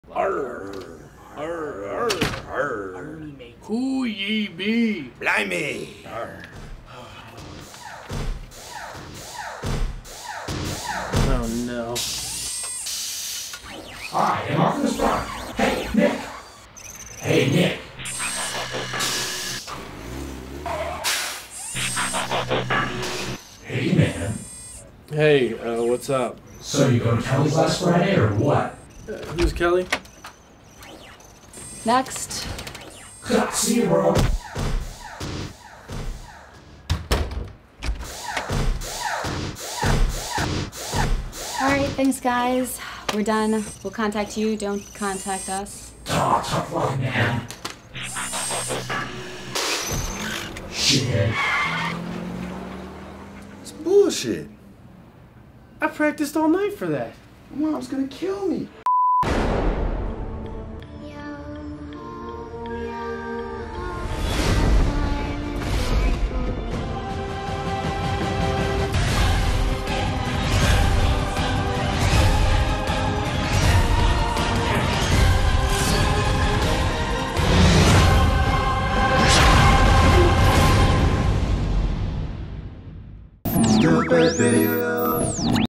Casting Optimus Prime
Optimus Prime shows up in a casting line for an upcoming film.  I guess the rumors were true.  He is kind of a jerk outside the studio.